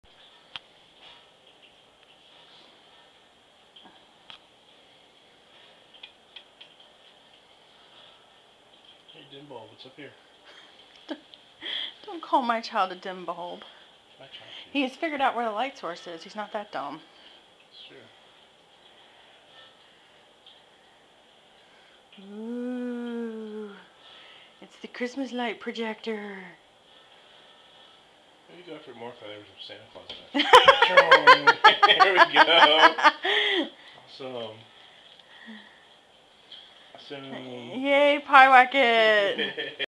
Pye spotlight
Our cat Pye, thinking the Christmas slide projector is his laser pointer toy, jumps for it!